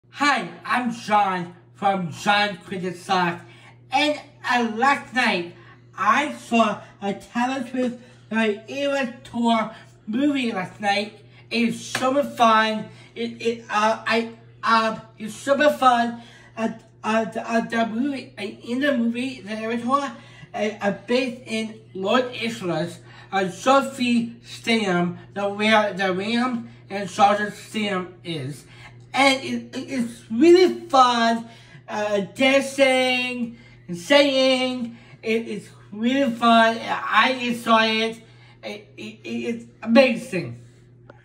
Taylor Swift: The Eras Tour Movie
John saw the Taylor Swift movie and wanted to share is reviews!